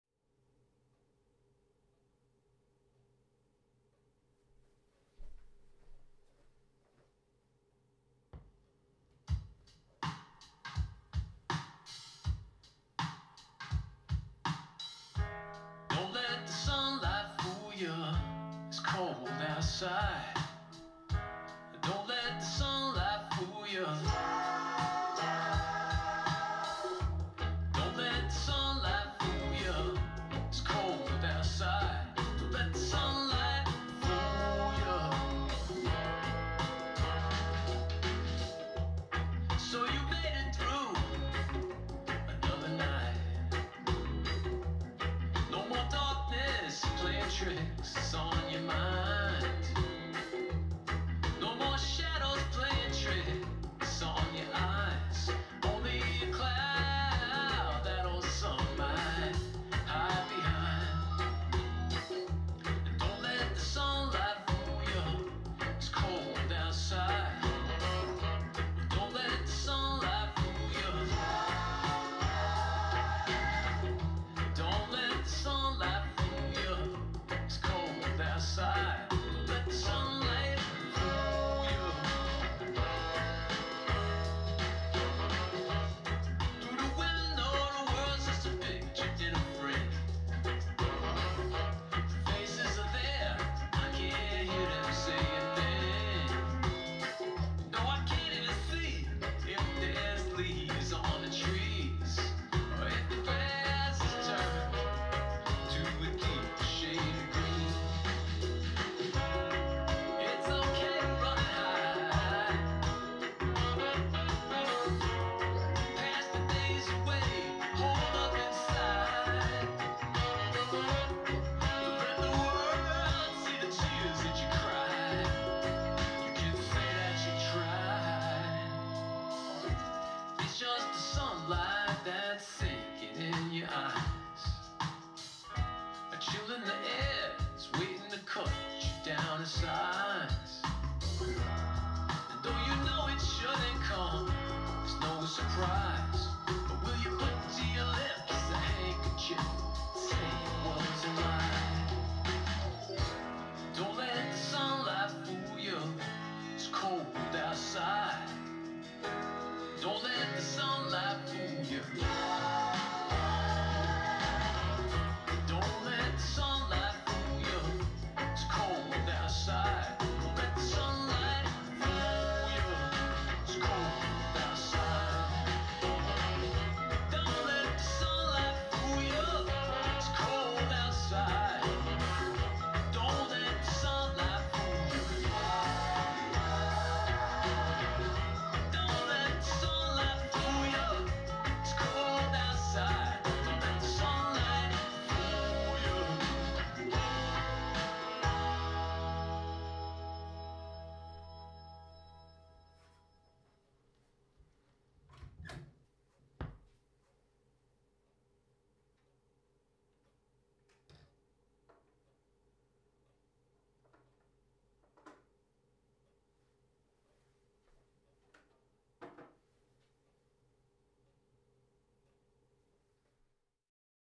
A nice Kenwood stereo rig with JVC record player and Philips speakers
Not exactly the best choice for an audiophile, but if you want to stack it nicely and have some speakers that are of a reasonable size, this is not a bad experience! Also, yes; That's the speakers that came with the freebee audio rig. CD player shat on itself. It ded.